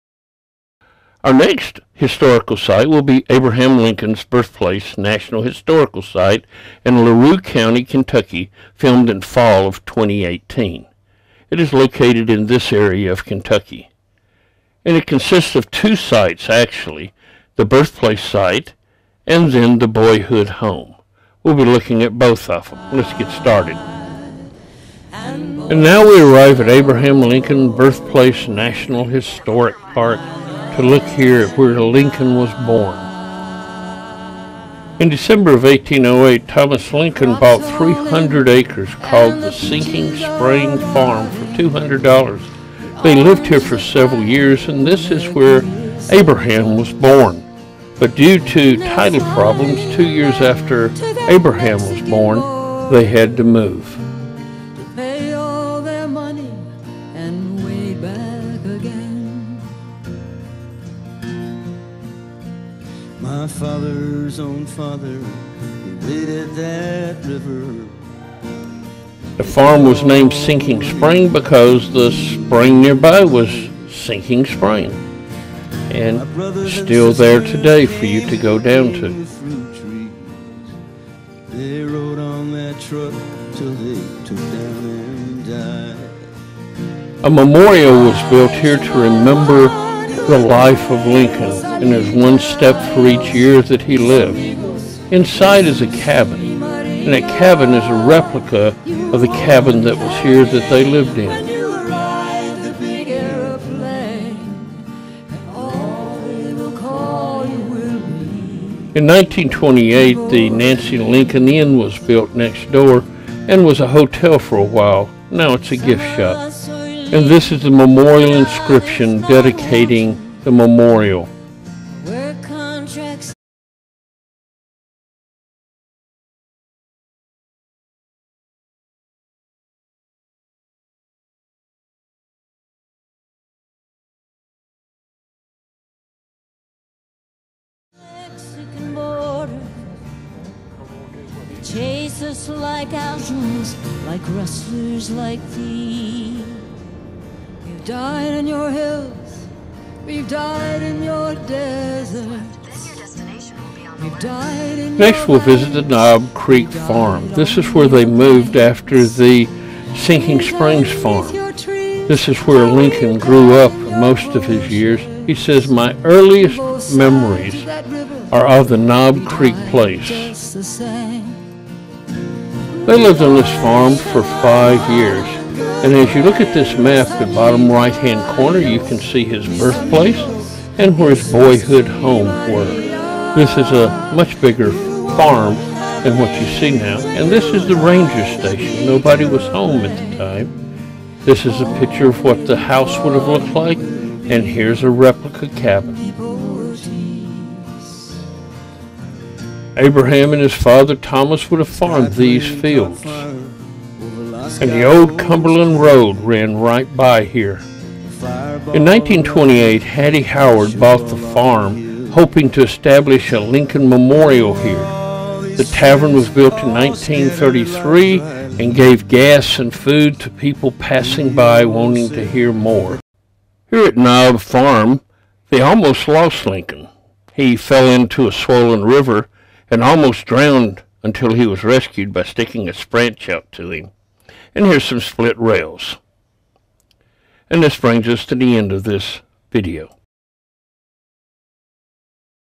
Abraham Lincoln Birth Place Historical Park
This is the second of this series on the National and State Parks and Historical Sites in the USA we have been to.  It covers his birth place and also the farm he lived on for several years.